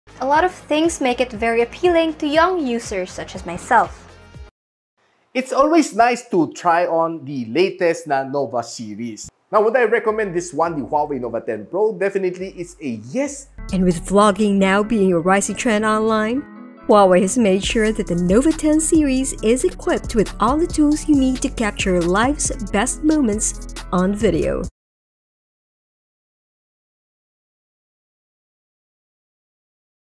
Hear What The Tech Media Love About the HUAWEI nova 10 series
Hear what the tech media in the Philippines have to say about the all-new HUAWEInova10seriesPH!

Pre-order the bestvlogphone now, starting at P17,999!